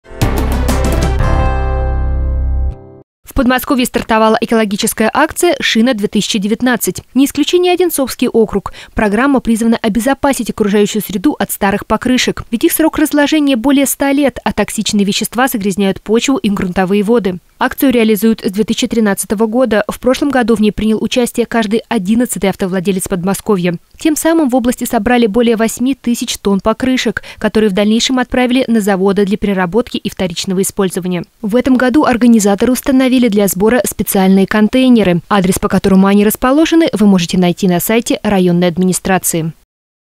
Операция «Шина»
- В Одинцовском округе стартовала экологическая акция «ШИНА–2019», призванная обезопасить окружающую среду от старых покрышек.